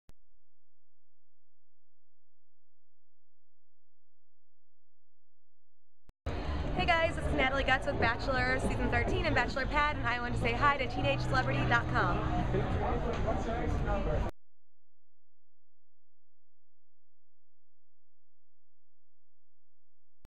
Natalie Getz TeenageCelebrity.com
Natalie Getz says Hi to TeenageCelebrity.com at the BenchWarmer.com Event hosted by  BWDPR.com  and celebrity Jessica Hall!